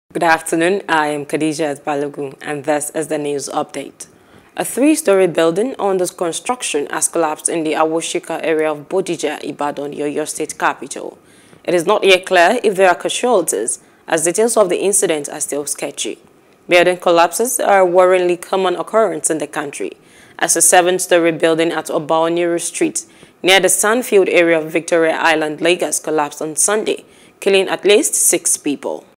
Three-Storey Building Collapses In Ibadan | NEWS
A three-story building under construction has collapsed in the Awosika area of Bodija, Ibadan, the Oyo State capital.